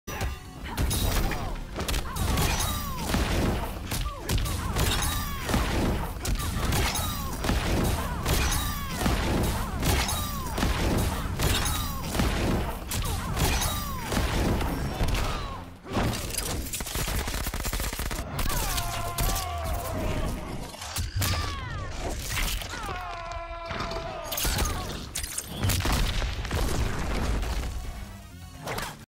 Could you imagine??
If NRS ever decided to give a character a 15 second unlimited  breakaway trait Spawn is gonna go nuts